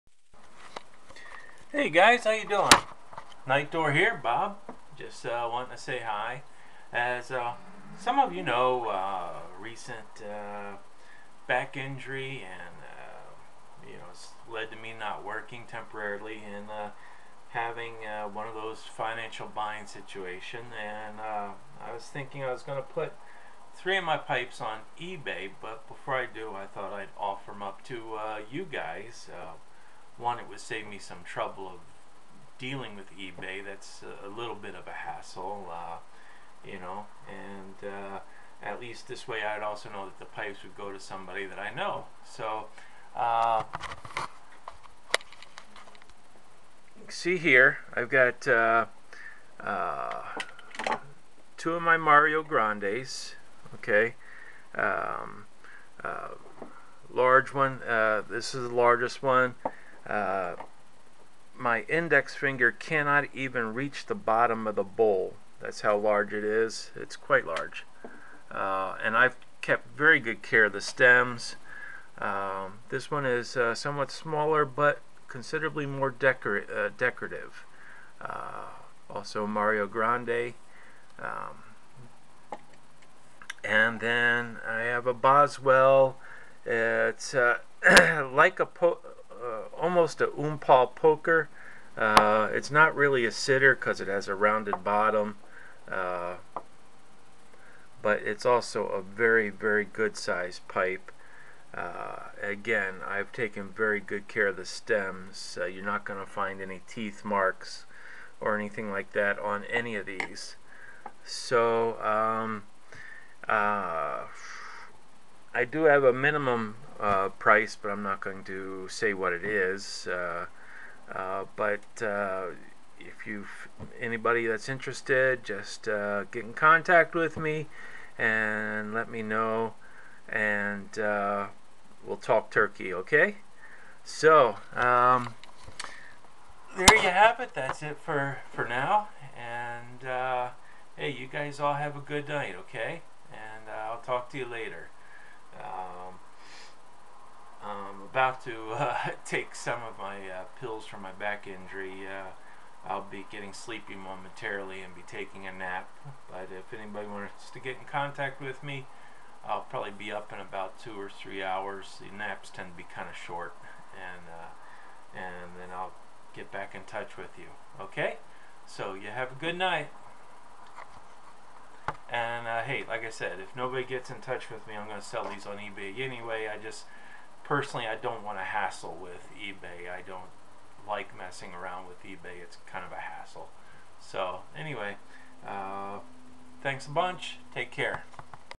Selling off 3 of my pipes
Hard times from being out of work Im selling off 3 of my pipes. Ive taken good care of these pipes none have teeth marks on the stems and all have been kept very clean. please make and offer